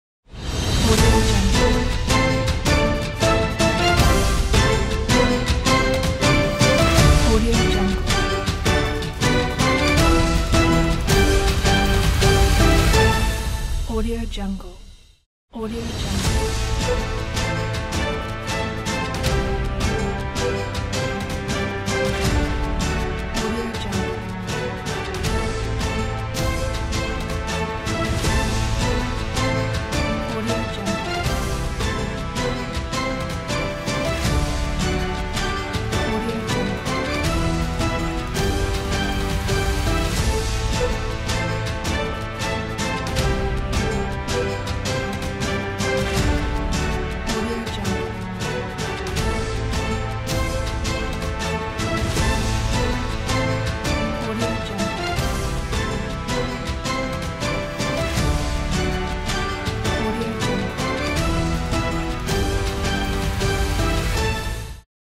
Full News Broadcast Package by IceVizion | VideoHive - After Effects Templates
The News Broadcast Package After Effects template is the perfect project for a news opener in Full HD resolution. This amazing news package features pre-rendered 3D scenes and is editable with After Effects CC2014 and higher. This template is easy to use. Open it up, change the text, video or image, and render. Creating a professional news intro does not get any easier. Tutorial included. Music and video not included. Font: Bebas Neue